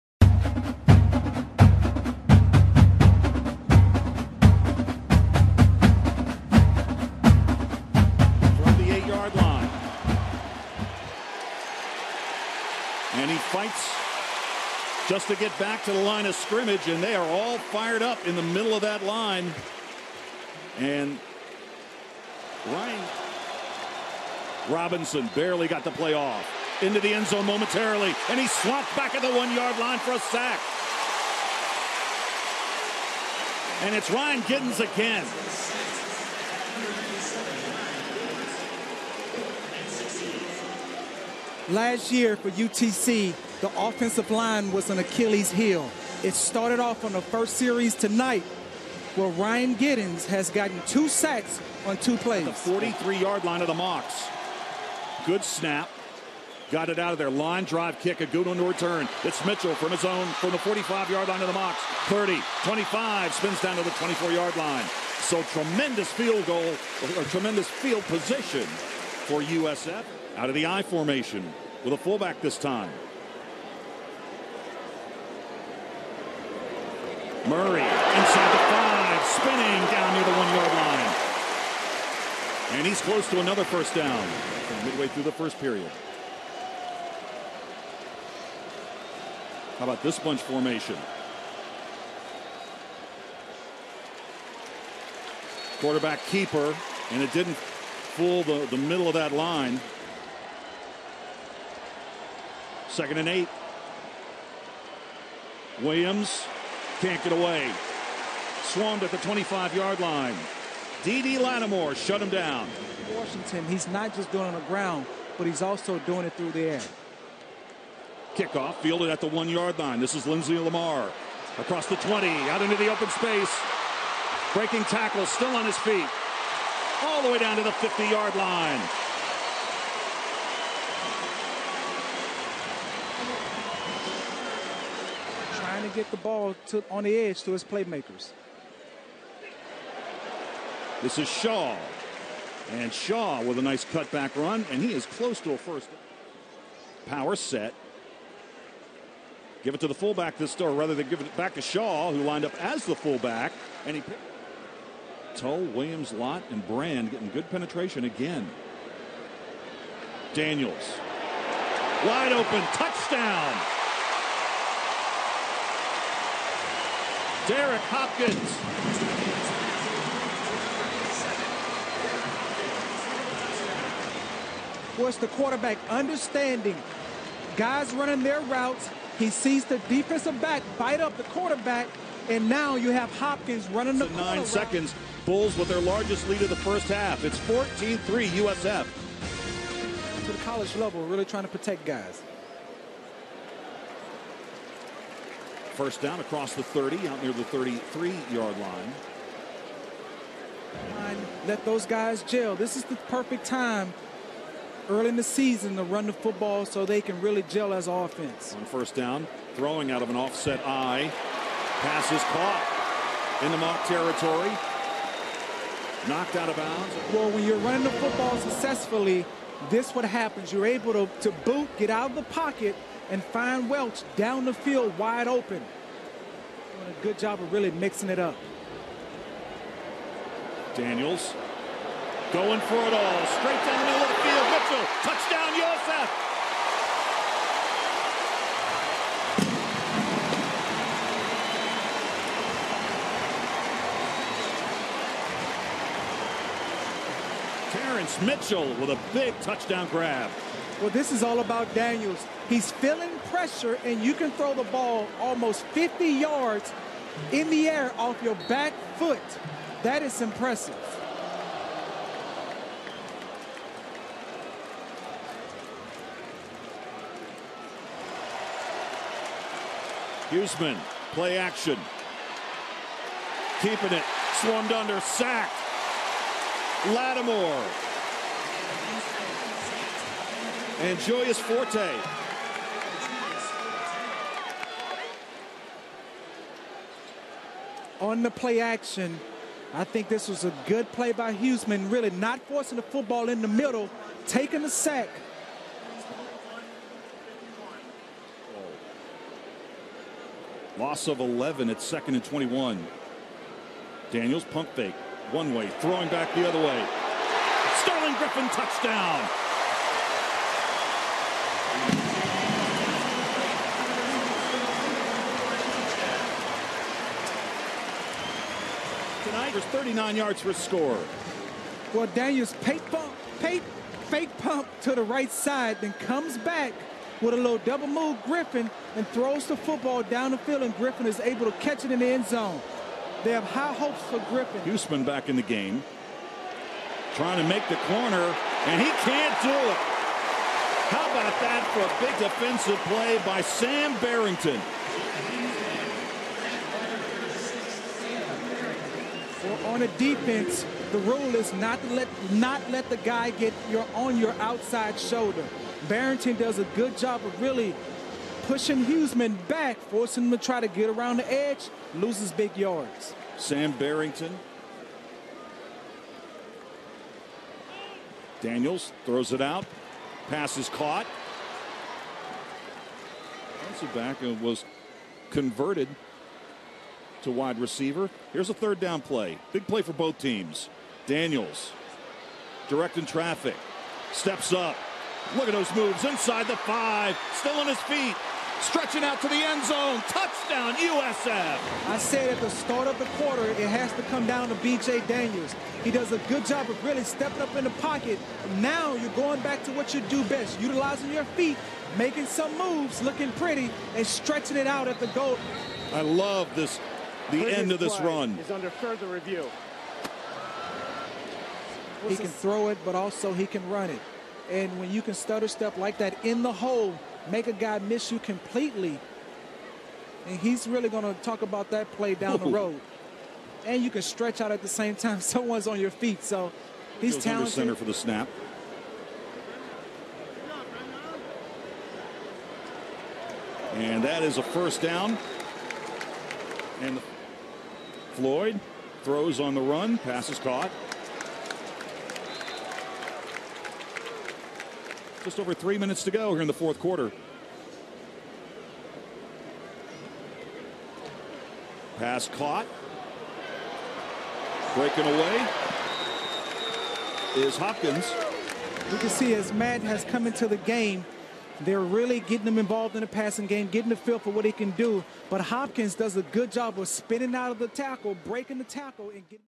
USF vs. UT-Chattanooga Highlights 2012
Highlights of the matchup between the University of South Florida Bulls and the University of Tennessee at Chattanooga Mocs.